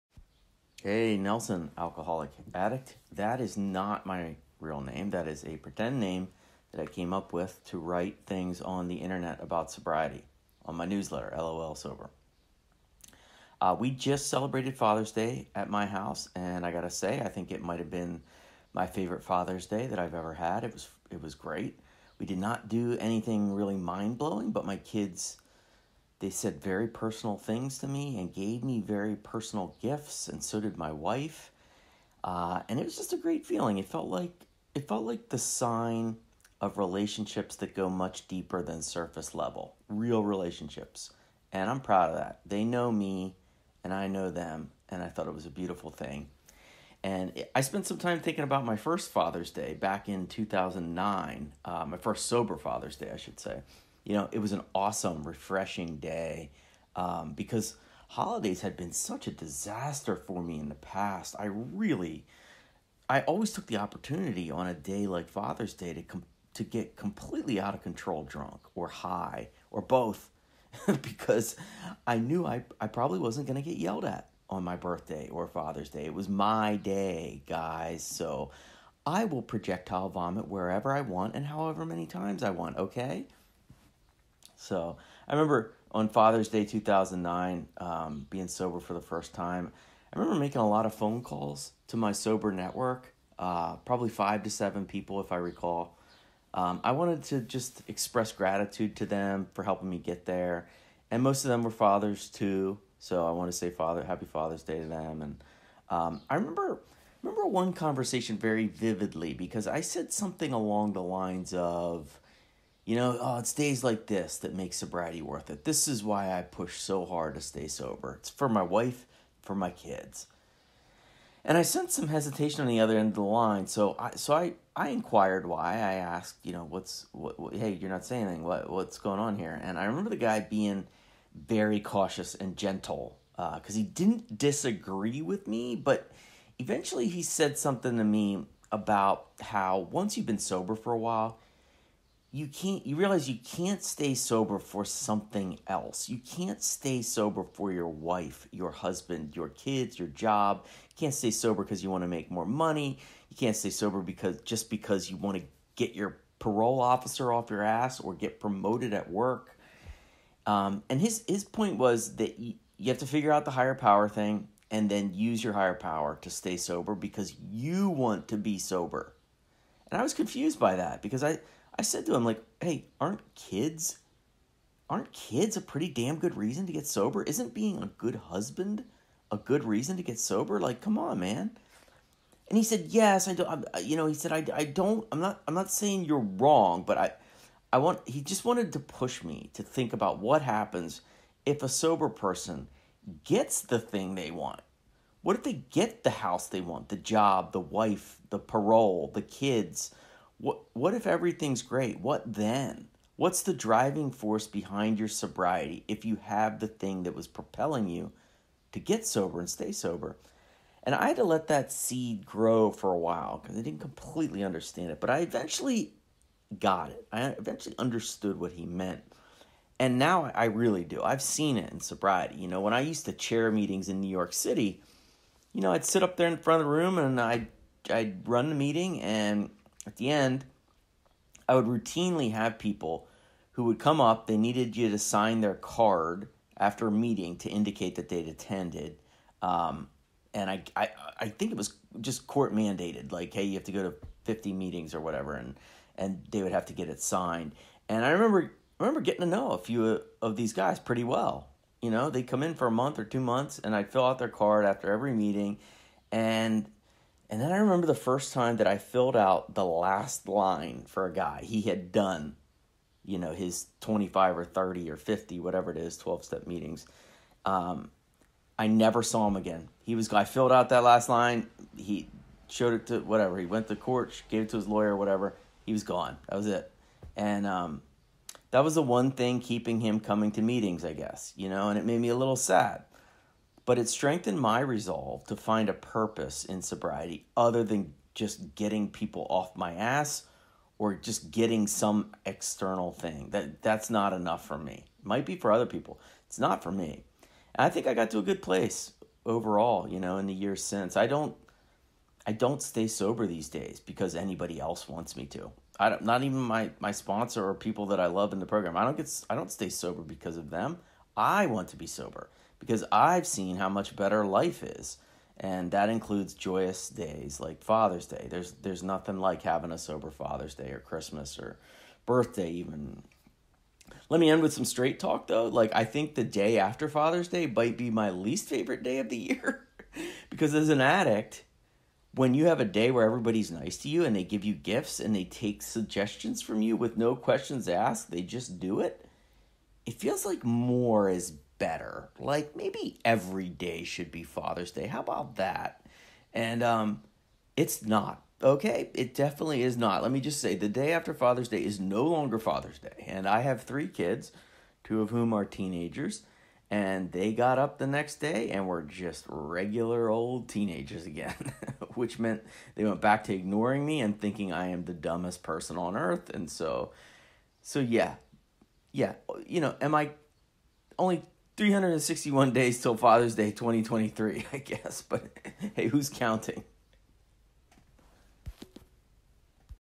The Father's Day predicament
Yet another realization that I can't stay sober just because I am a dad. (For more sober humor, please check out NelsonH.substack.com)